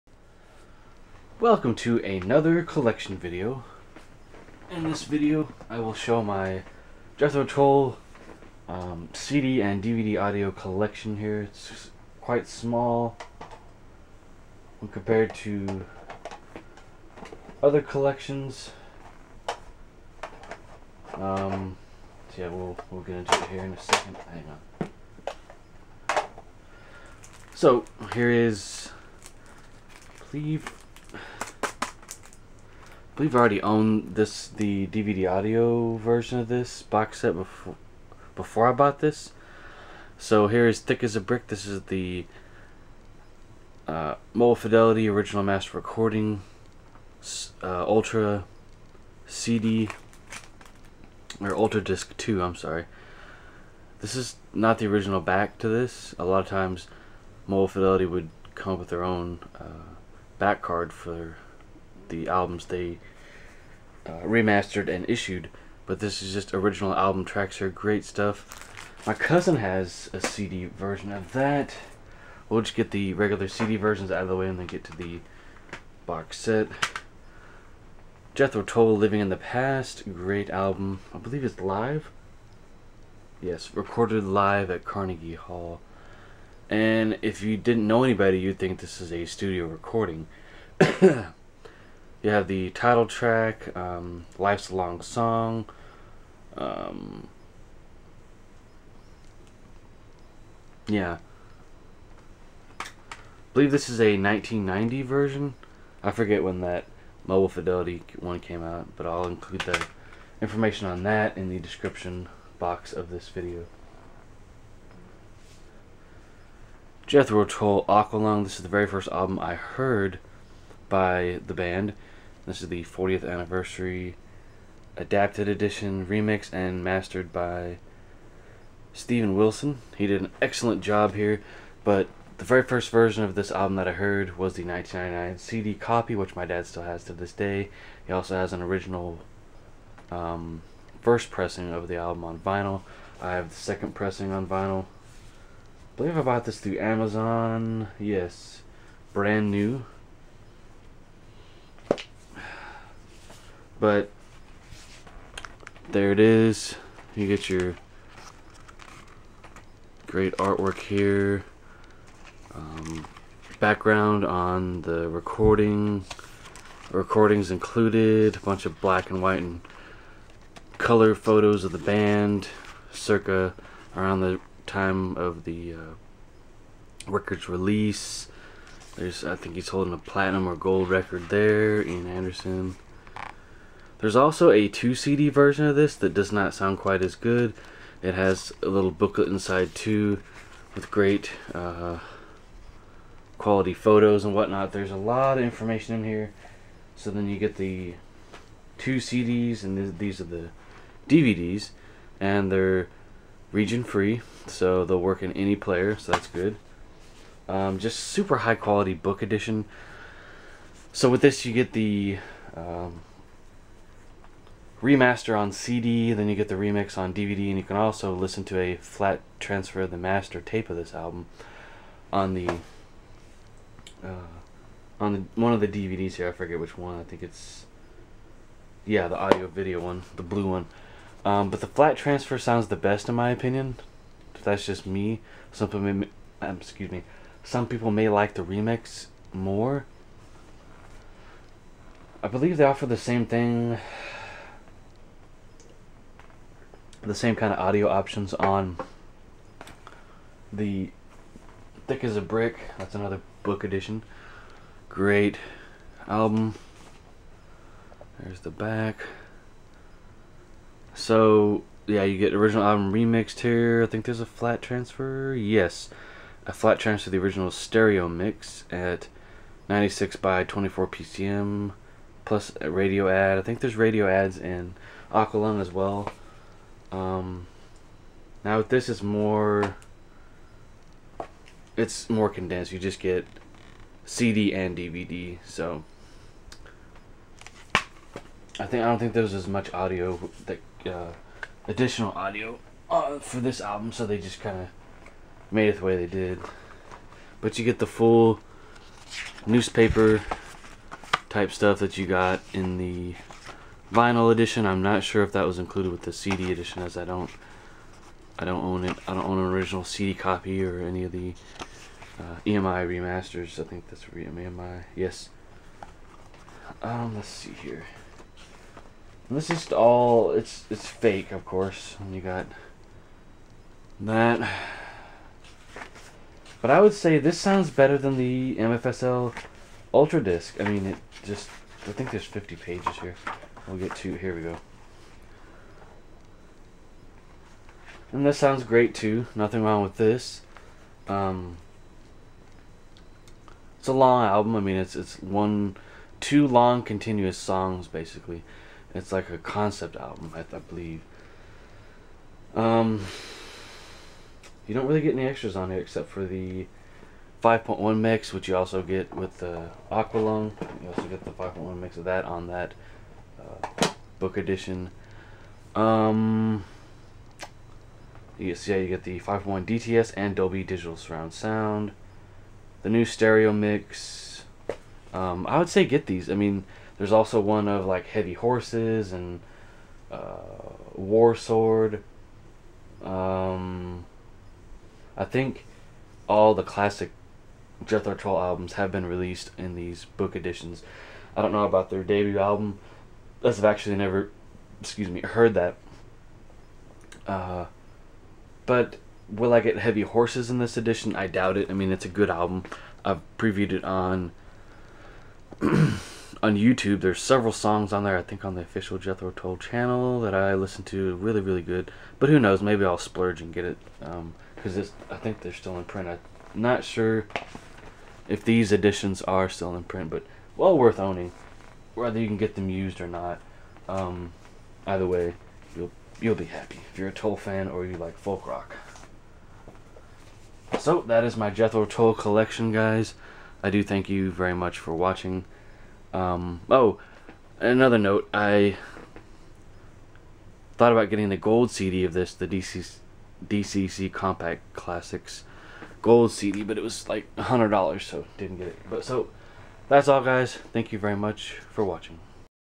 Jethro Tull CD Collection
This is my Jethro Tull CD collection as of me posting this video. I might buy more JT CD's in the future if i can find them for a bargain.